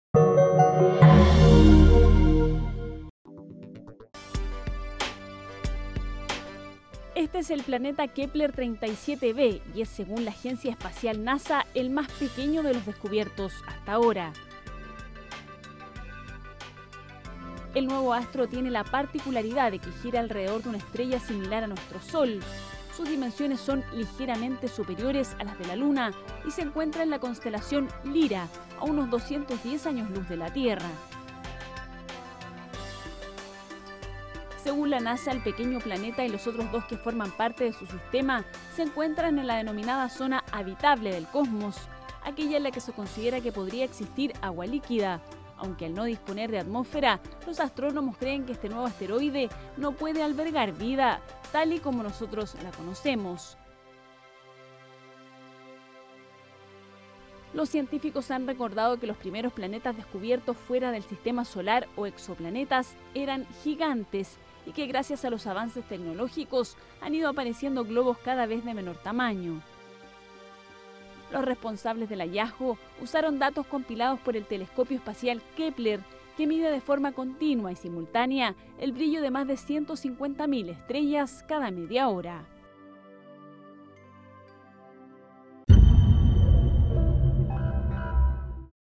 Descubren un nuevo planeta
La agencia espacial NASA informó del hallazgo del planeta más pequeño que gira alrededor de una estrella similar a nuestro Sol